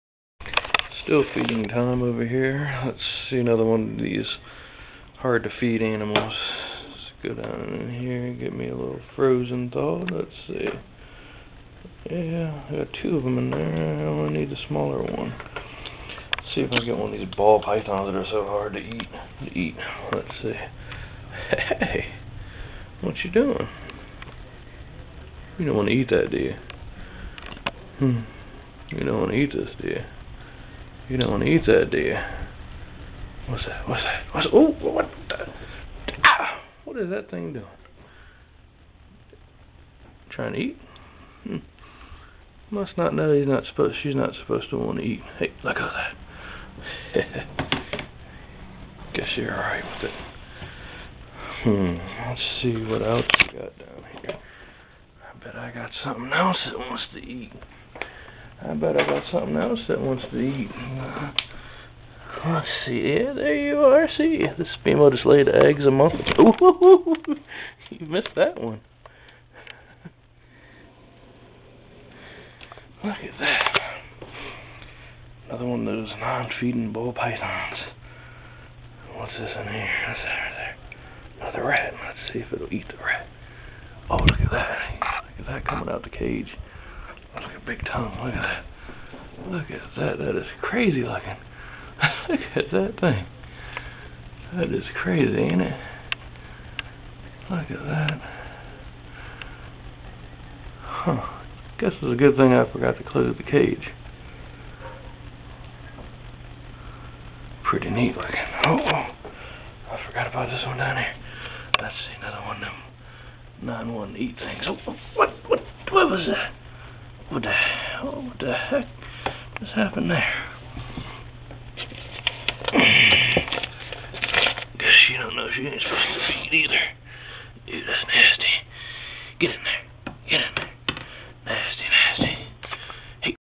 Ball and Blood eating
Here is a couple of Balls and one of my Bloods eating shes at the end..